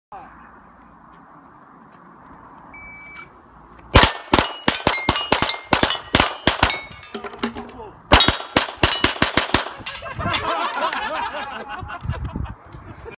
【极限军事俱乐部】日常IPSC靶场Stage练习 25 趣味射击小比赛 4
日常IPSC靶场Stage练习 25 趣味射击小比赛 4  非常欢乐